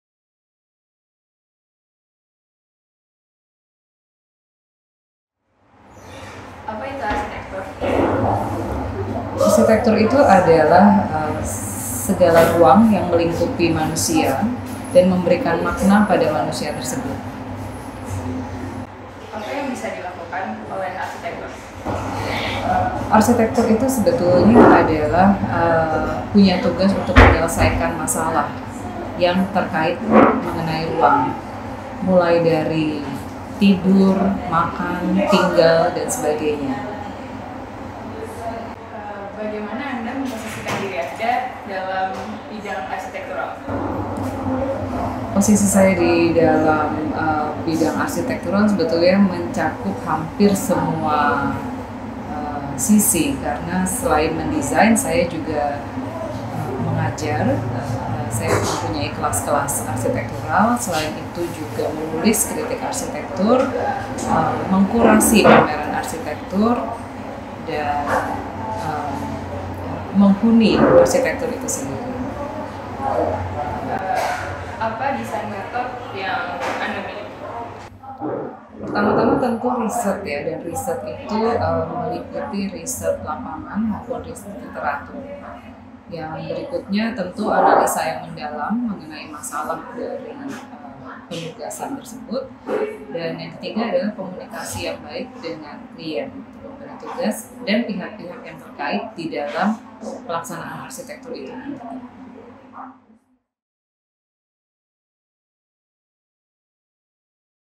Avianti Armand: What is architecture?
“Architecture is every space surrounding the human being and can give a meaning to it.” - Avianti Armand

What is architecture?
What can architecture do?
What is your architectural position?
What is your design method?

In the interview: Avianti Armand
Camera and interview: Ariefina Marta Rahmavani, Nashrisaf Muhammad Resal & Muhammad Ridhwan Yusfianda